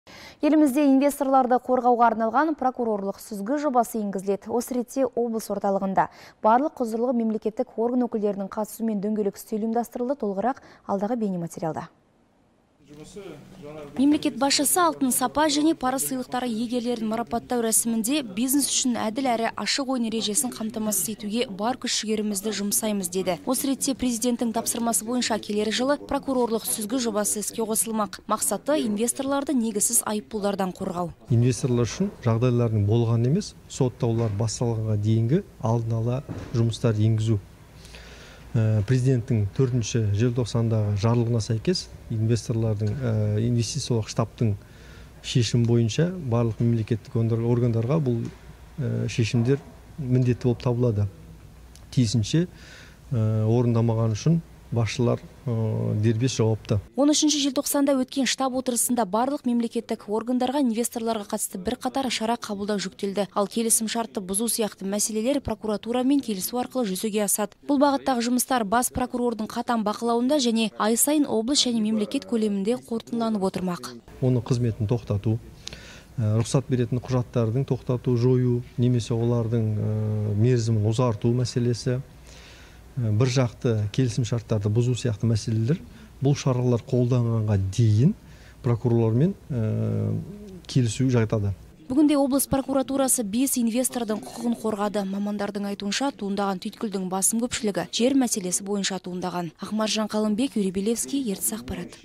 Келер жылы «Прокурорлық сүзгі» жобасы іске қосылады
Елімізде инвесторларды қорғауға арнаған «прокурорлық сүзгі» жобасы енгізіледі. Осы ретте облыс орталығында барлық құзырлы мемлекеттік орган өкілдерінің қатысуымен дөңгелек үстел ұйымдастырылды. Толығырақ алдағы бейнематериалда.